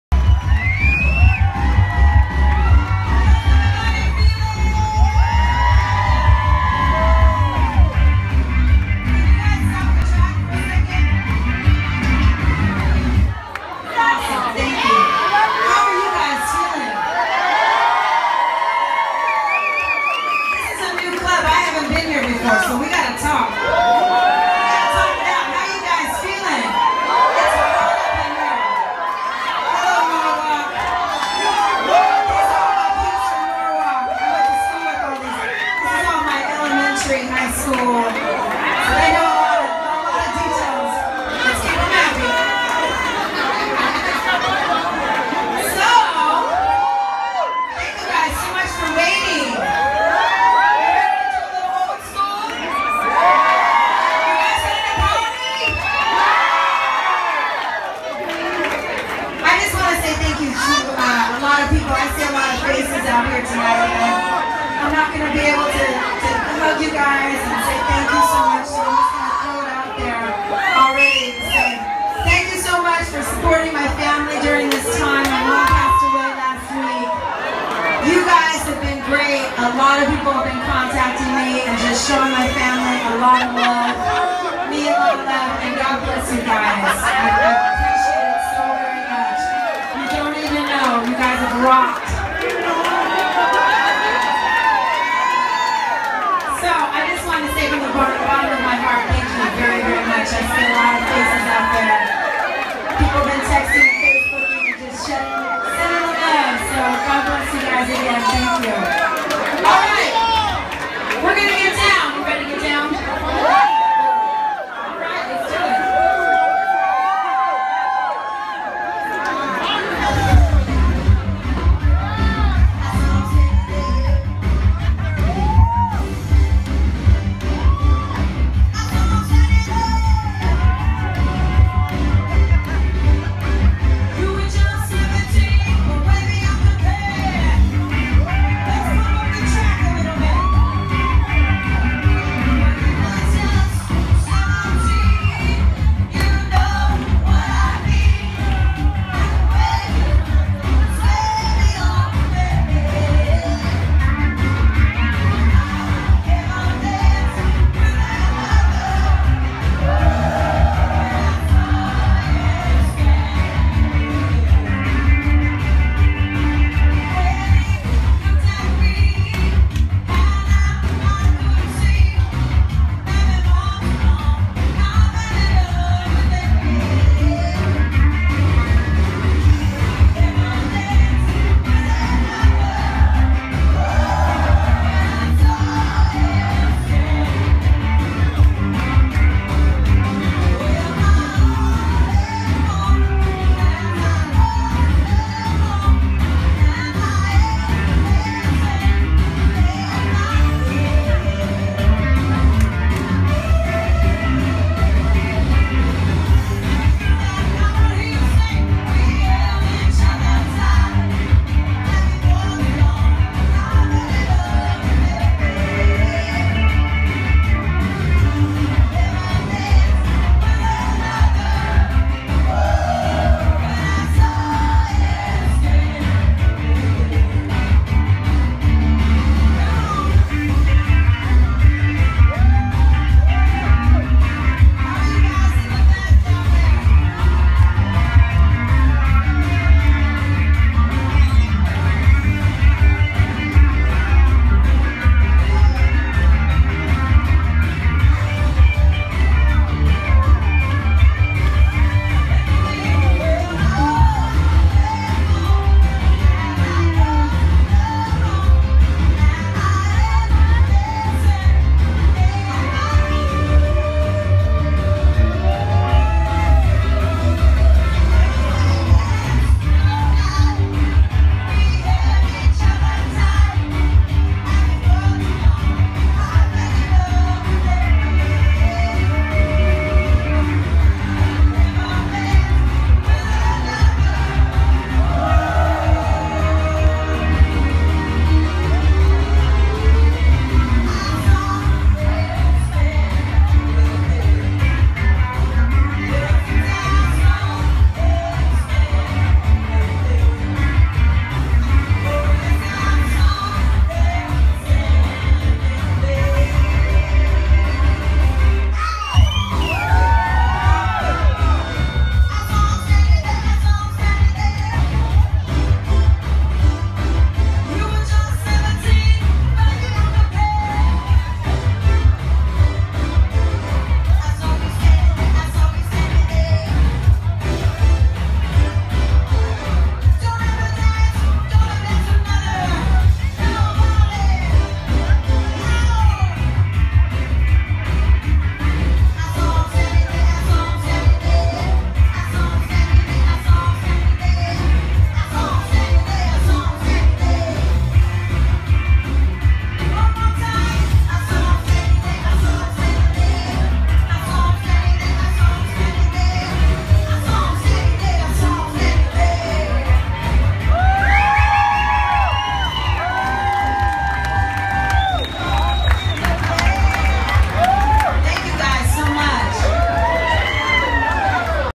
Tiffany signing her first song at the totally 80s bar and grille!
This is Tiffany singing her first song at the totally 80s bar and grille!!!